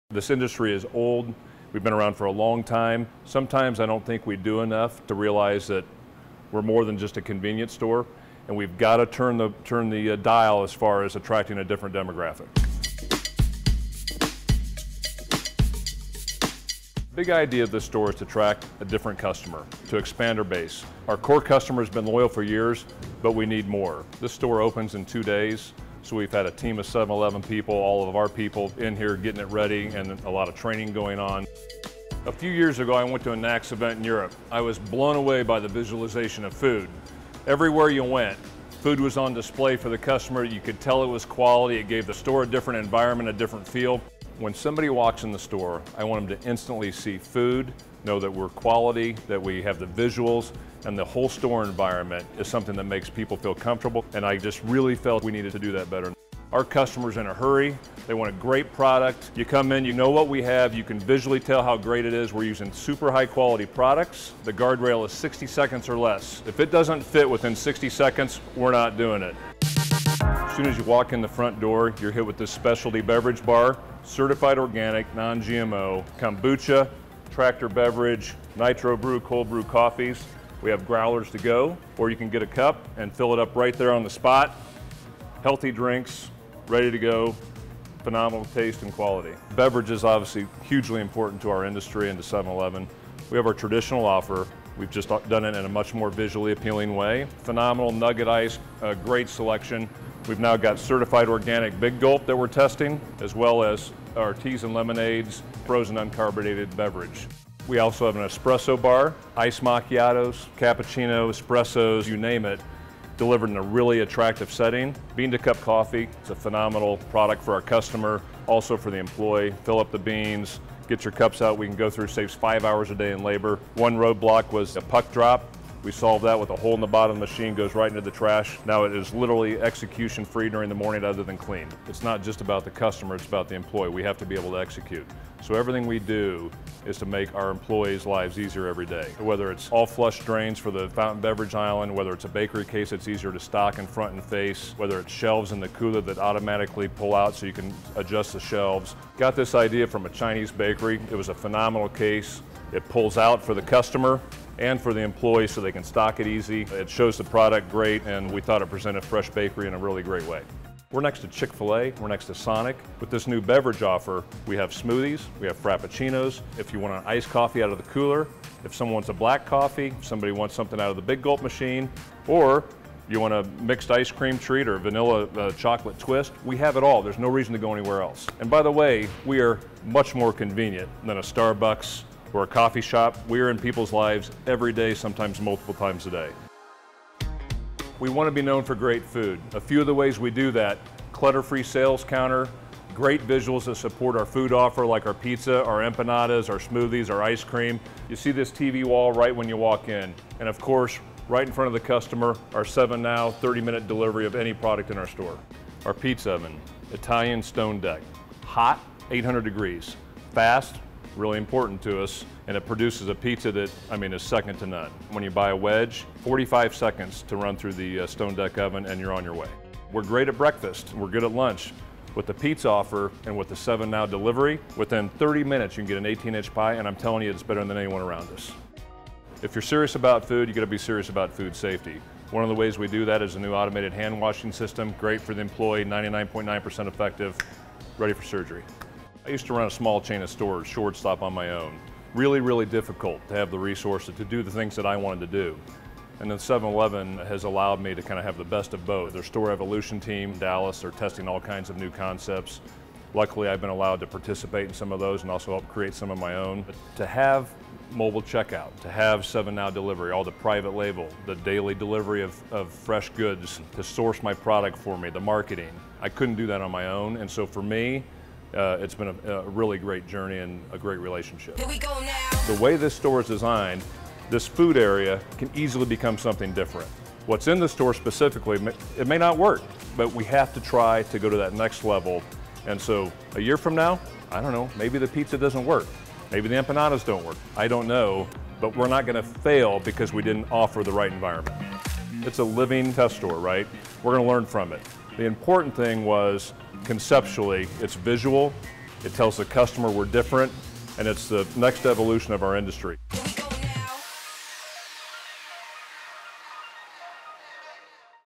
Ideas 2 Go: Retail Evolution
Former NACS Chairman Dave Carpenter, president and CEO of J.D. Carpenter Companies Inc., opened a new 7-Eleven store in August 2019 just outside of Denver, inspired by the visualization of food that European retailers create with their fresh and prepared food offers and displays. The store features unique elements that reach new demographics and elevate the overall industry definition of convenience